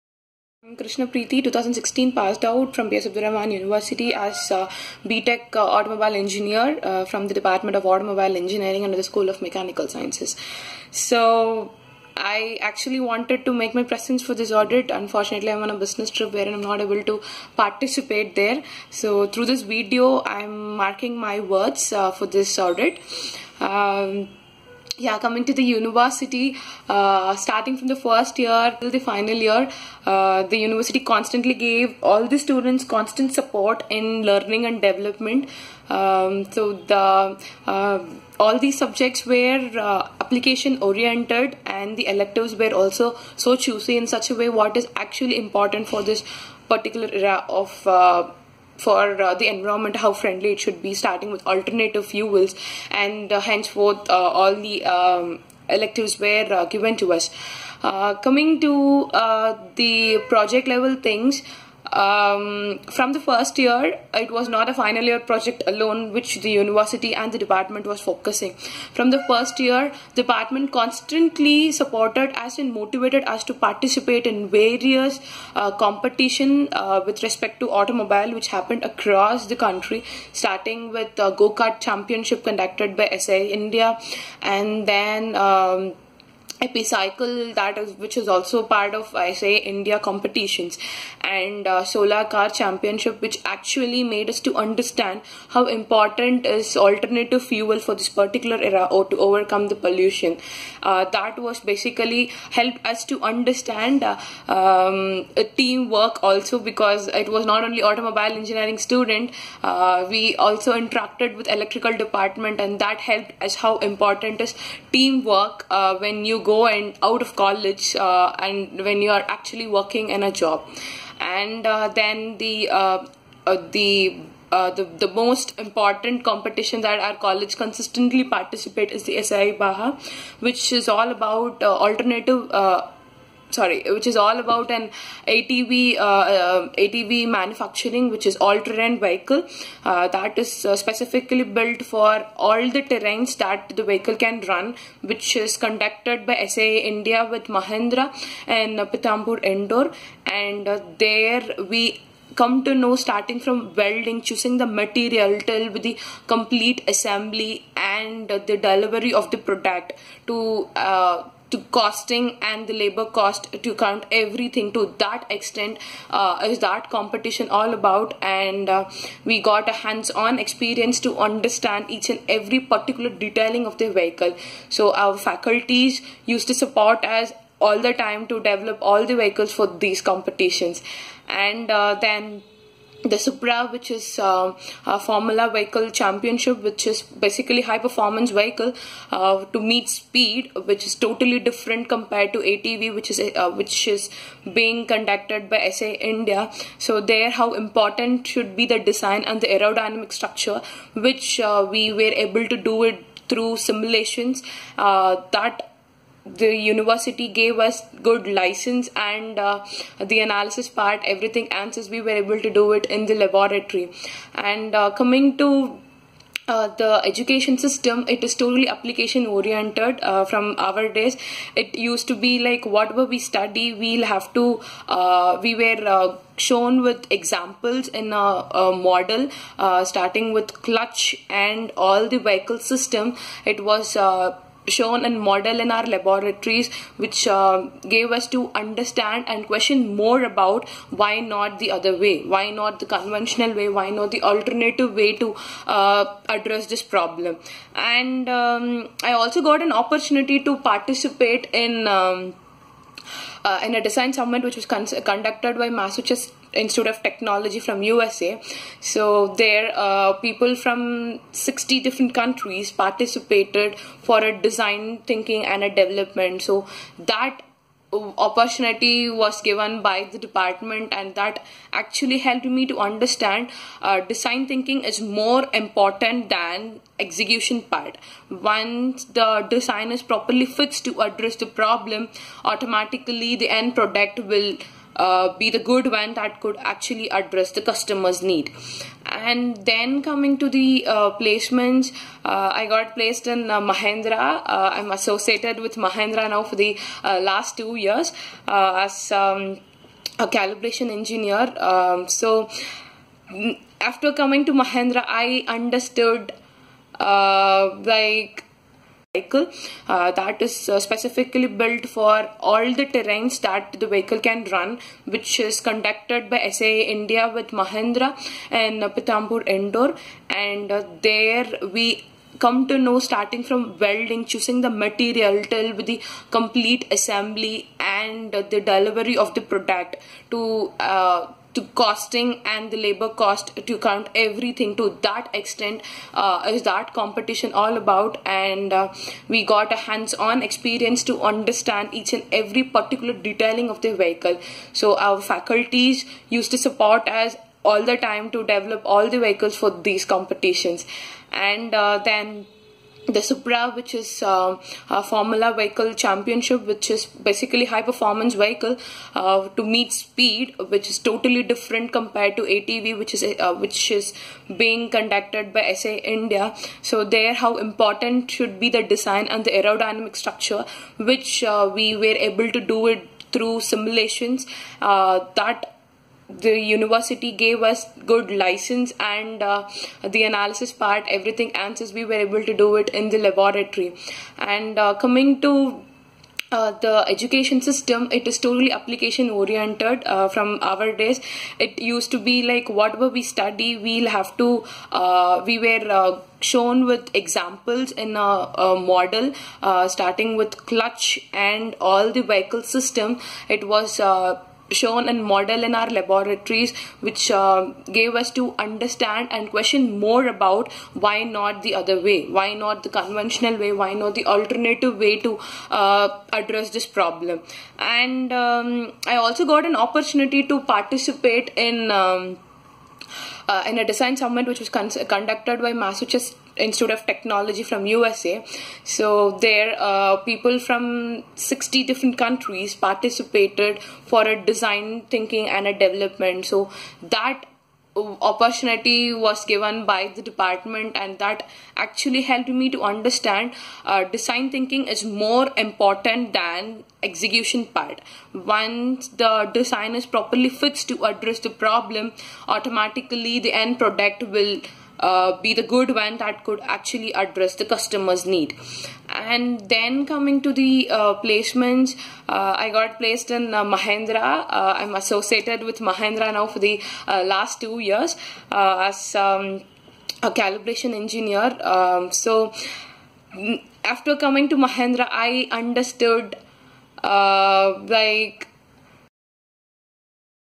Alumni Testimonial - B S A Crescent Institute of Science & Technology
Krishna Preeti - B.Tech Automobile Engineering
2016 Passed out Student.

The department of Automobile Engineering was started in the year 2011. This is the youngest and rapidly developing department in the institution. The department is having young faculty members guided by experienced professors.